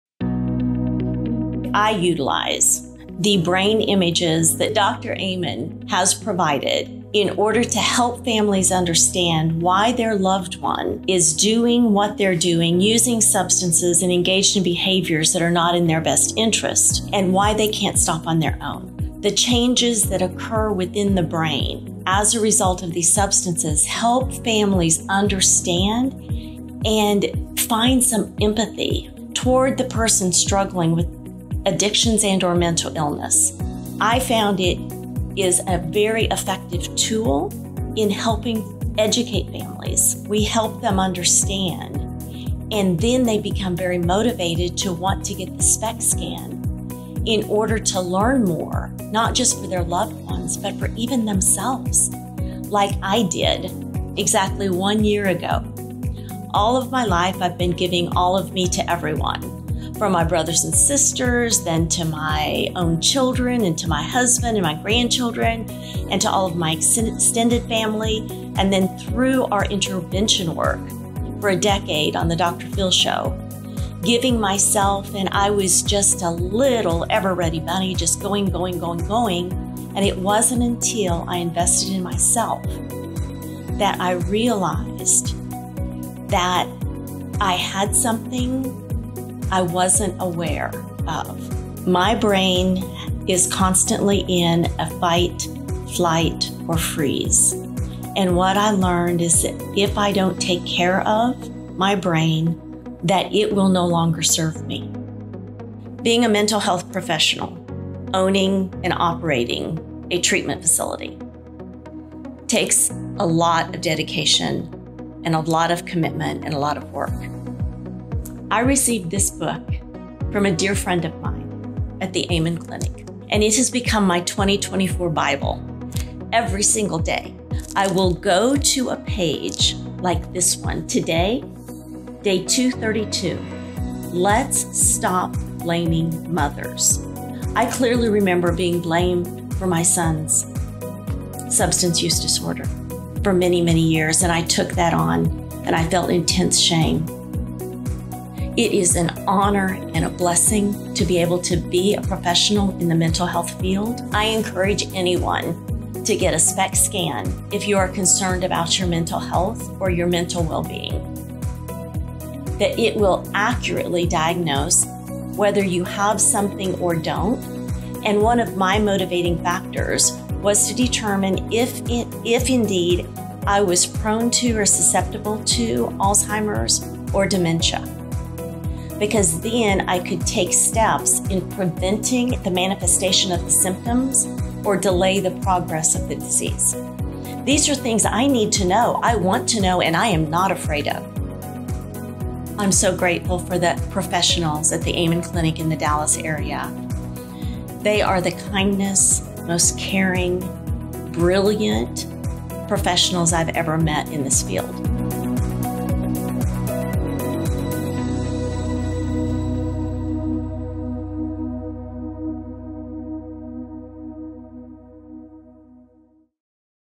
The Role of Brain Imaging in Substance Abuse Recovery
Treatment and recovery for people with a history of substance abuse can be tricky. But as Touchstone Ranch Recovery CEO Debra Knauss has discovered, there are tools that can not only increase compliance and forgiveness, but also monitor and track progress. Chief among these tools is brain SPECT imaging. Debra describes how she came to partner with Amen Clinics, a psychiatric clinic that specializes in imaging and holistic treatments, in order to see a radical improvement in the success rate of her clients.